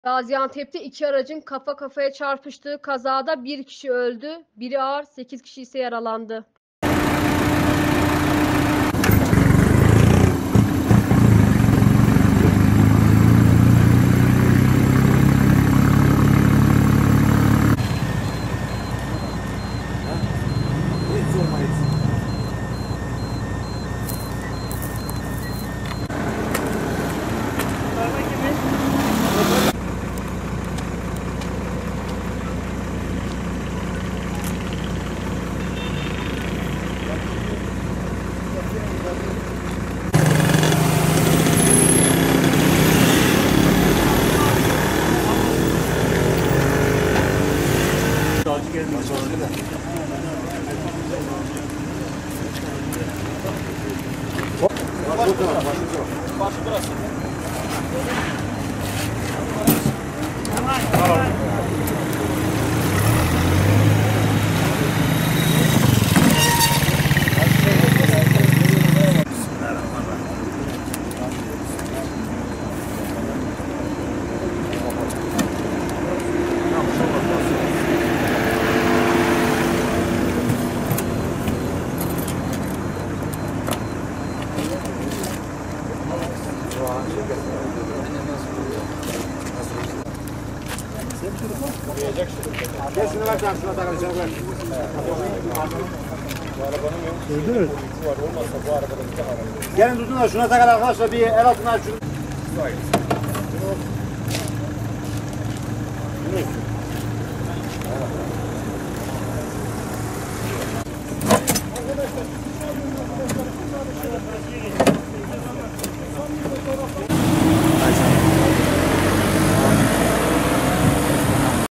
Feci kaza: 1 ölü, 8 yaralı
Hafif ticari araçlar kafa kafaya çarpıştı